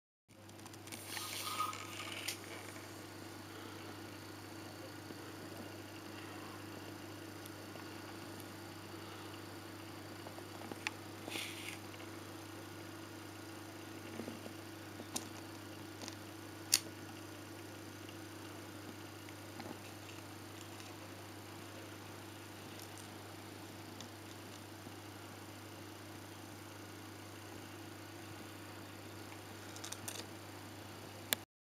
Isbre, Betta splendens macho "doble cola"
Isbre es un Betta splendens macho que lleva conmigo desde el 6-feb-2012